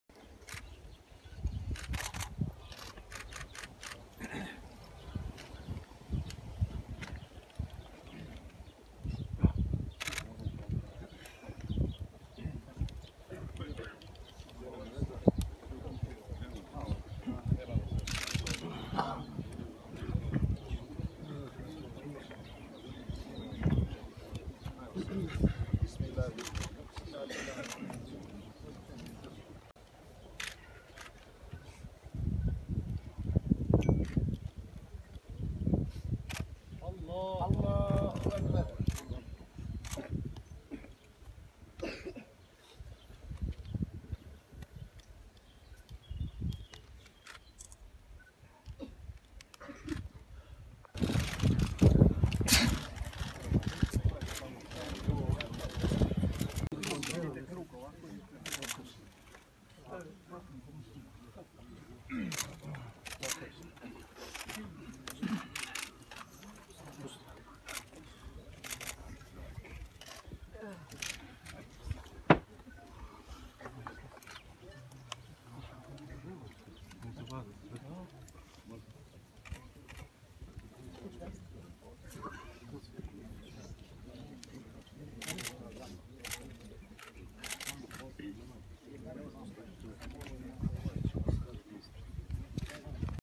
Sahrana Sinana Sakića
Folk legenda Sinan Sakić sahranjen je na groblju u Loznici.